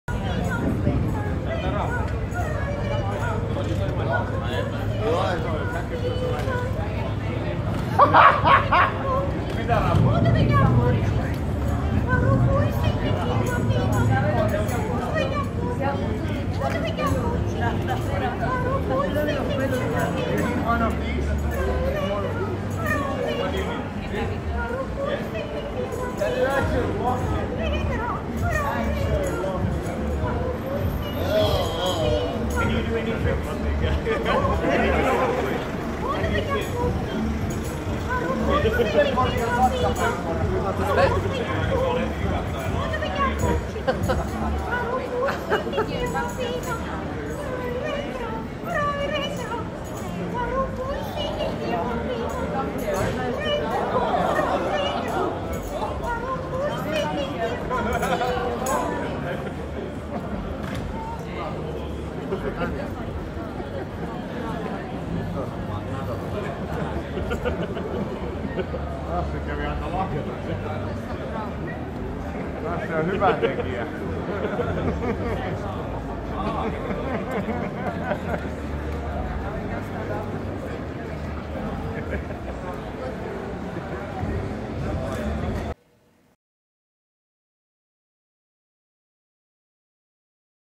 Bachelor Walking With Strict Mistress in Town
Some of best moment of Riga Stag Party Weekend with Strict mistress.